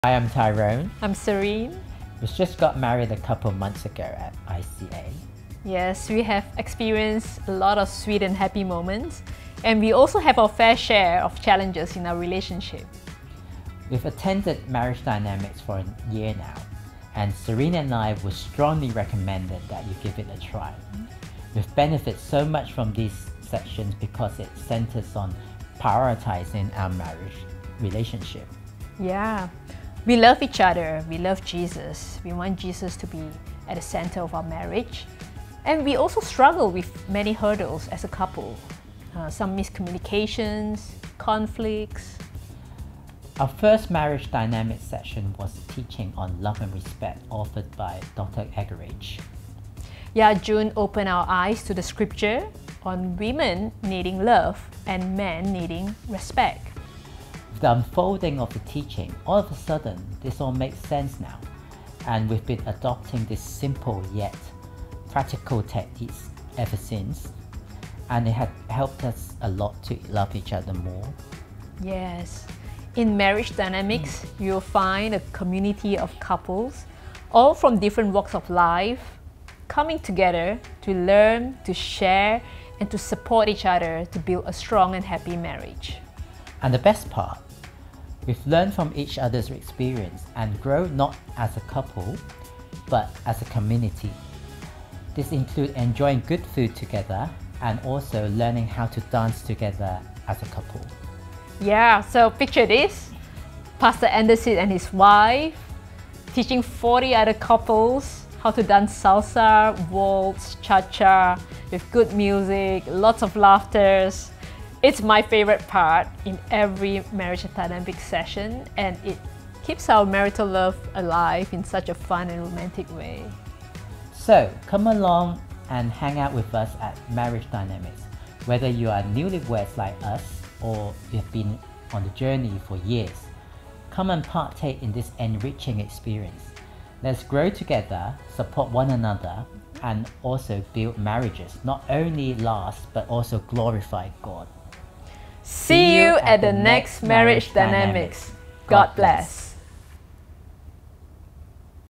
Serene & Tyrone's Story
Check out Serene and Tyrone's story and how Marriage Dynamics has helped them as newlyweds! For more info on Marriage Dynamics, head

ICA HK (International Christian Assembly, Hong Kong)